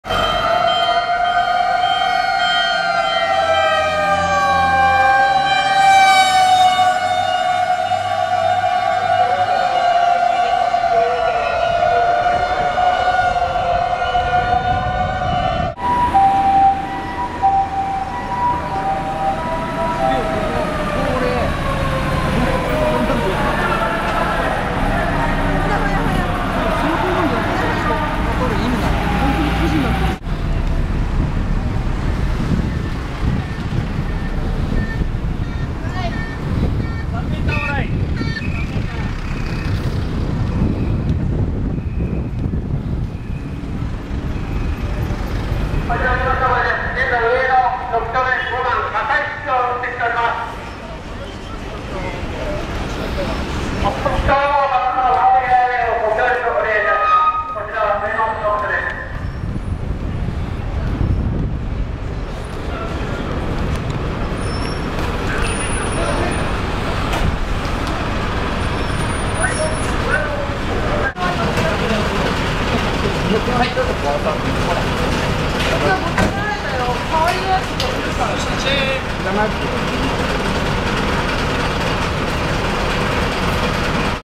火災現場へ激走する消防車。fire trucks arrival.
中央通りを激走する神田１と神田はしご。火災通報現場で消防隊確認するも、火災の事実はなかった。